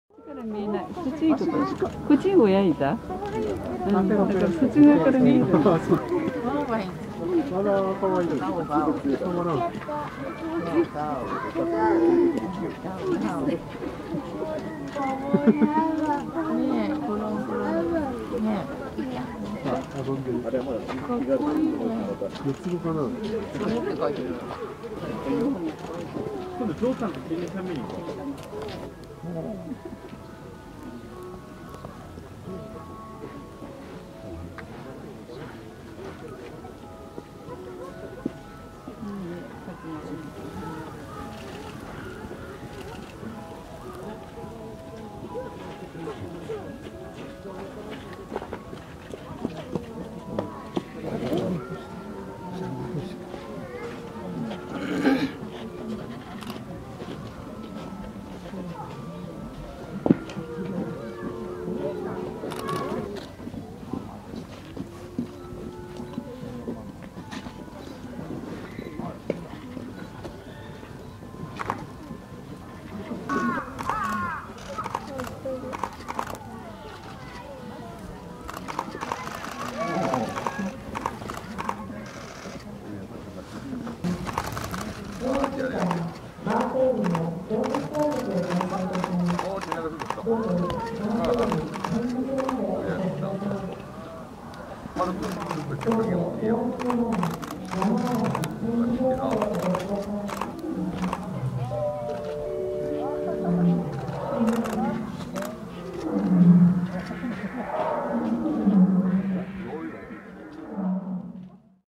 2020/01 ママはあったかい休憩所 in 秋田市大森山動物園
ドラちゃんが腰を降ろすと、わらわらと仔たちが集まってきます。グルーミングしてもらったりよじ登ったり。仔たちの身体がだいぶ濡れてきました。

カサンドラ♀：2014年5月25日生まれ。2016年3月ノボシビルスク動物園より来園。
令(れい)No.7910♂：2019年9月29日生まれ。父ヒロシ 母カサンドラ。
風(ふう)No.1111♂：(同上)
和(なごみ)No.7575♀：(同上)
月(つき)No.5015♀：(同上)

(2020/01/12撮影)